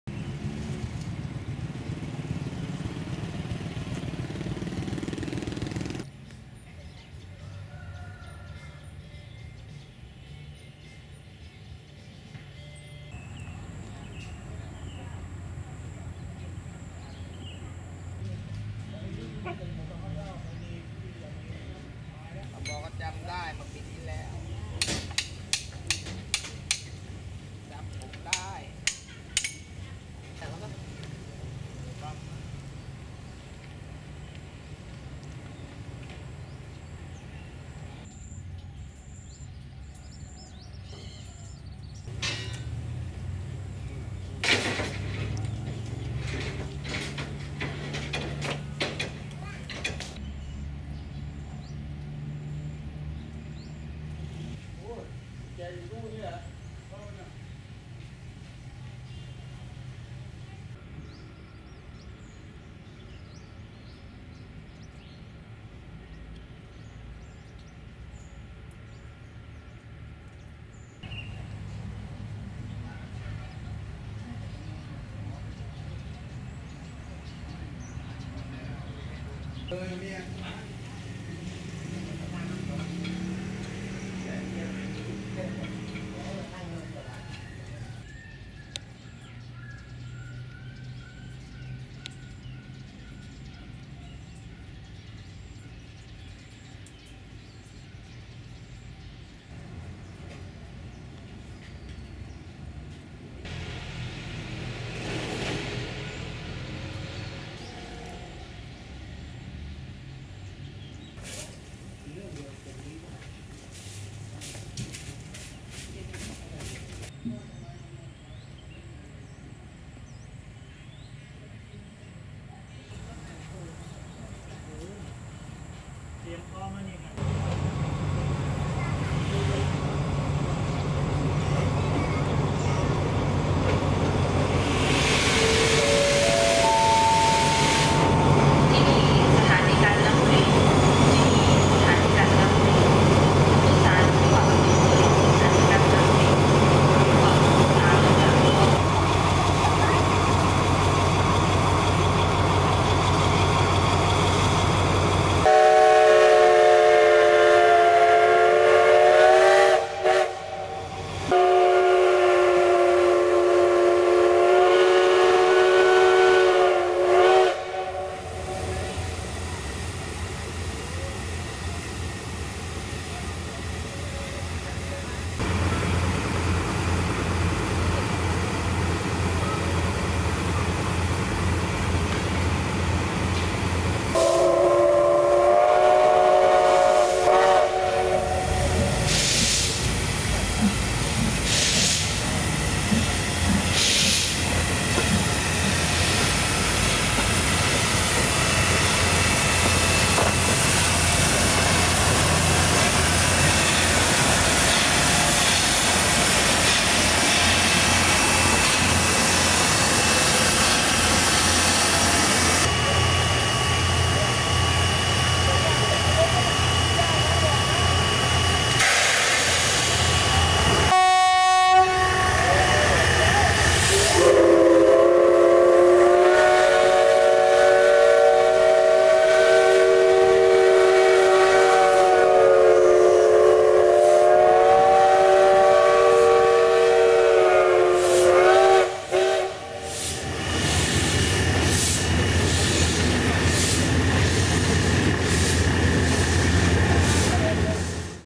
Steam Train at the River Kwai,sights and sounds
Japanese WW2  steam locomotive being prepared by engineers to cross the bridge on the River Kwai in Thailand. C5615 was built by Hitachi, Japan in 1935. It was used on the Death Railway in Kanchanaburi.